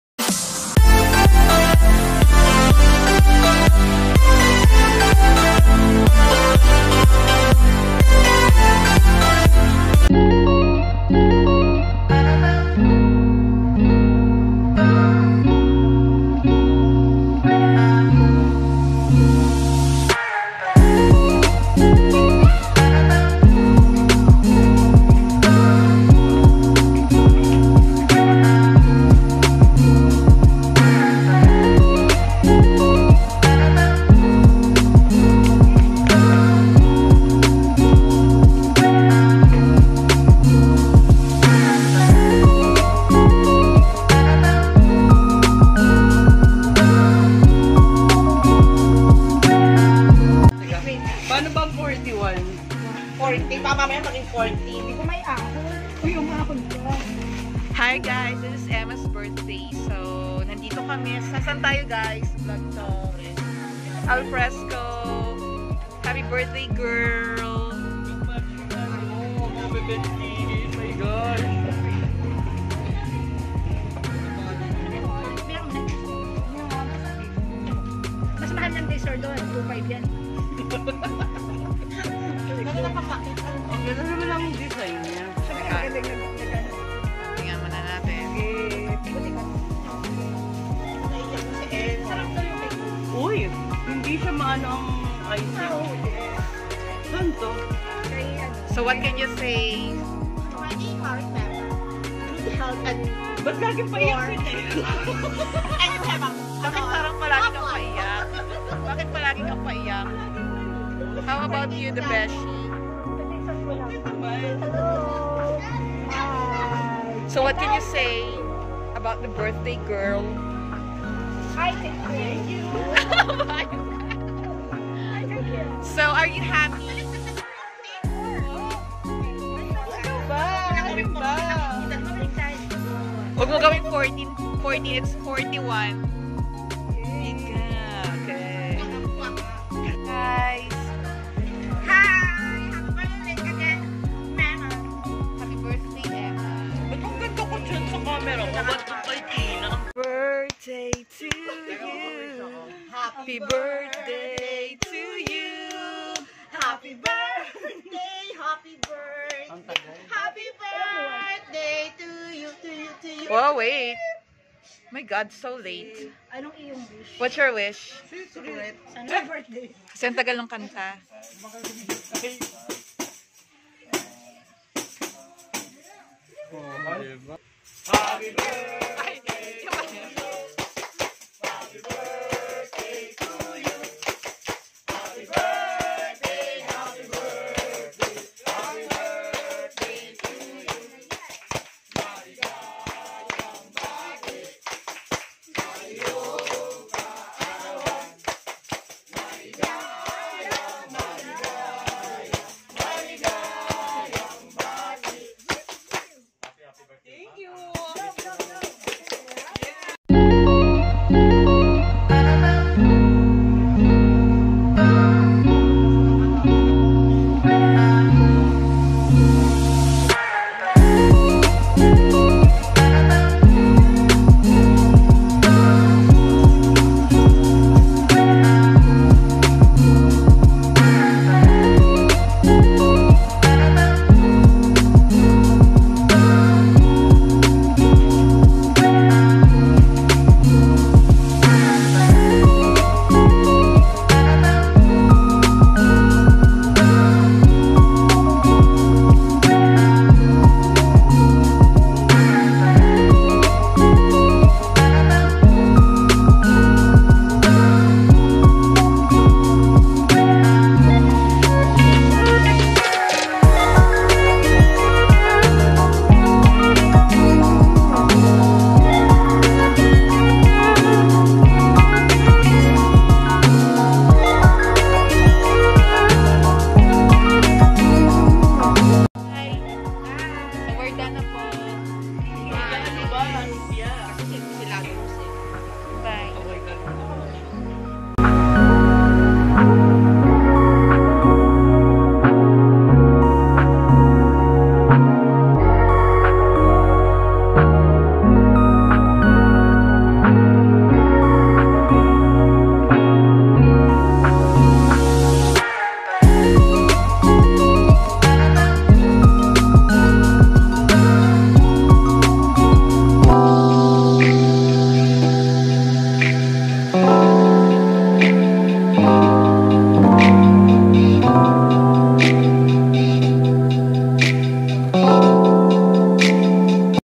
ALFRESCO JARDIN RESTAURANT RELAXING PLACE TO DINE IN (MY COUSIN'S SIMPLE BDAY CELEB)
Located at Tayabas Quezon